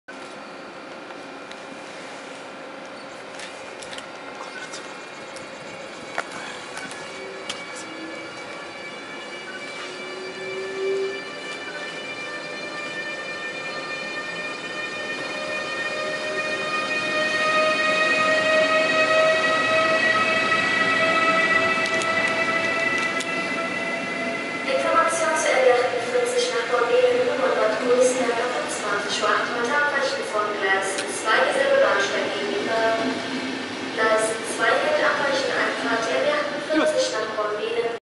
Roland Taurus 1216 955 mit AKE Rheingold in Bonn Hbf
1216 955 mit einem AKE Rheingold in Bonn am 2.1.2016